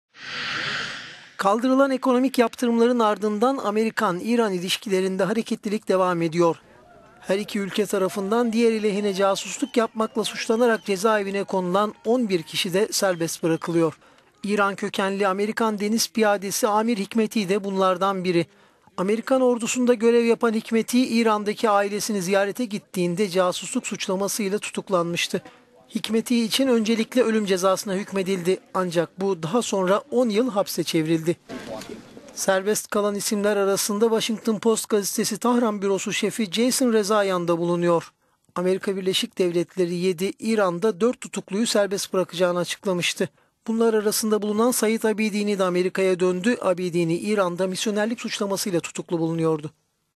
Casusluk suçu mahkumu 7 İranlı ve 4 Amerikalı serbest kalıyor
Kaldırılan ekonomik yaptırımların ardından ABD-İran ilişkilerinde hareketlilik devam ediyor. Her iki ülke tarafından diğeri lehine casusluk iddiasıyla tutuklanan kişilerden, serbest bırakılacağı duyurulan 11 kişi evlerine dönmeye başladı. ABD 7 İran ise 4 kişiyi serbest bırakacağını duyurmuştu.

Michigan'a özel jetle getirilen İran Asıllı Amerikan deniz piyadesi Amir Hekmati de Tahran'daki ailesini ziyarete gittiğinde ABD lehine casusluk suçlamasıyla tutuklanmış ve ölüm cezasına çarptırılmıştı.…